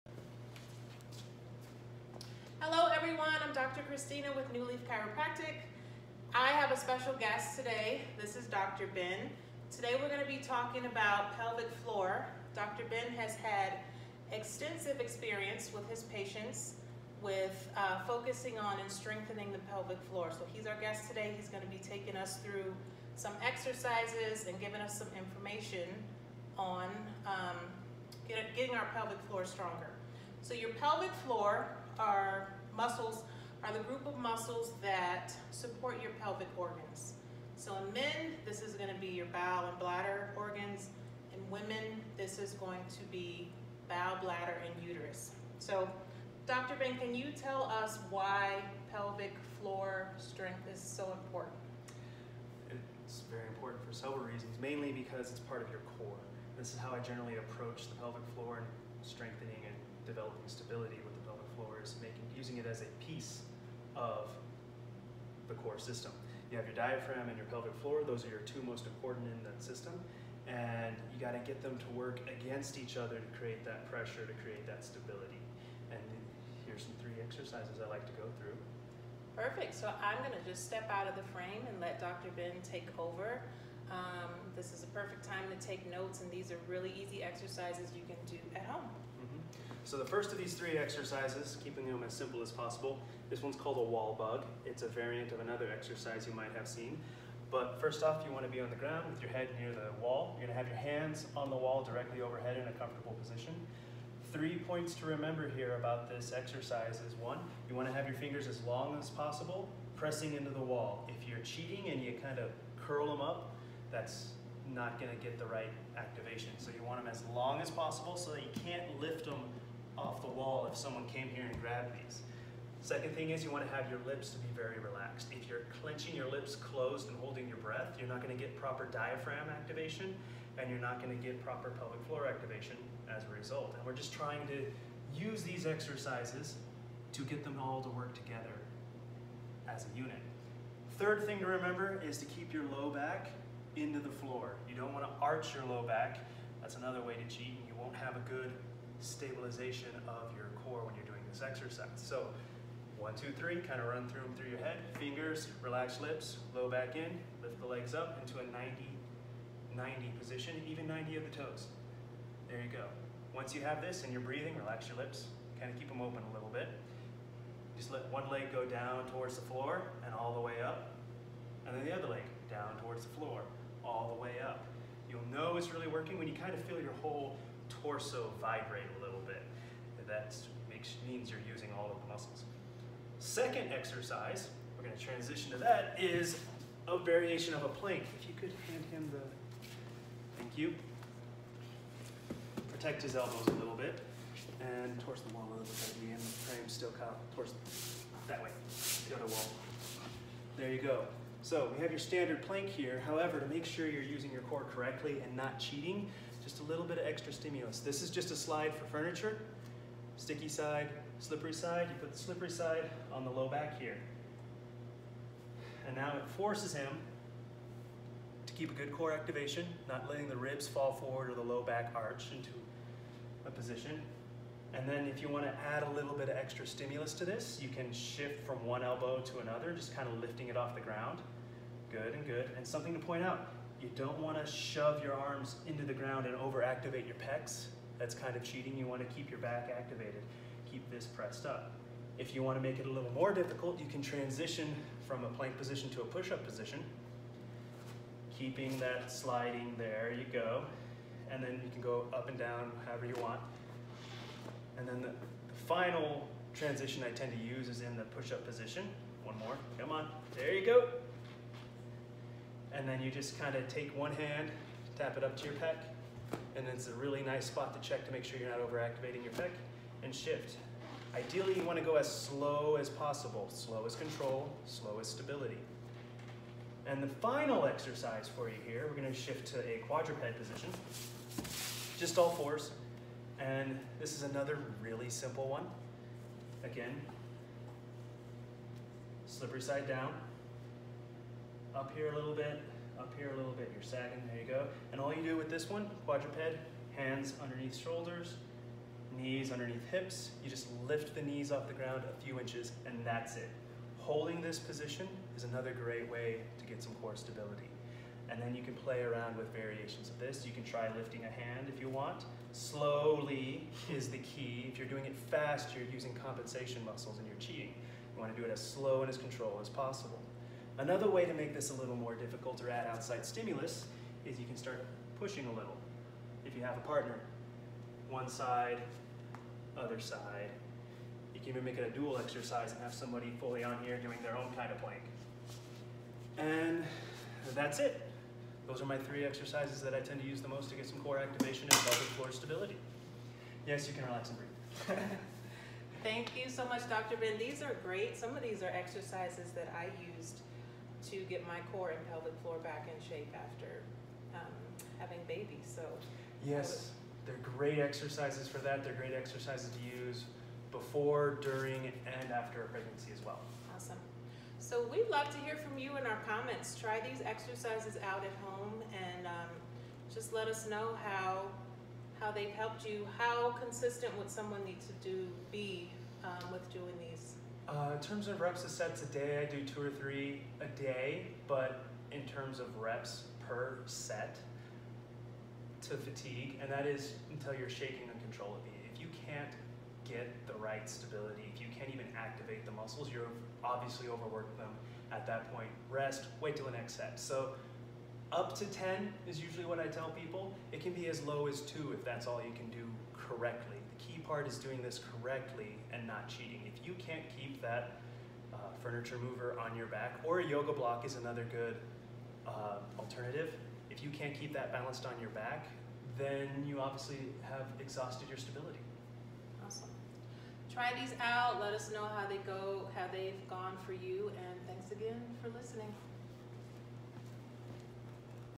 Core and Pelvic Floor Stability - For Men and Women
Longmont Chiropractors Dr. Christina Hunter, and Drs Kyle & Ben Buck talk about how a strong pelvic floor can help with core strength, low back flexibility/strength, and overall better health for your spine.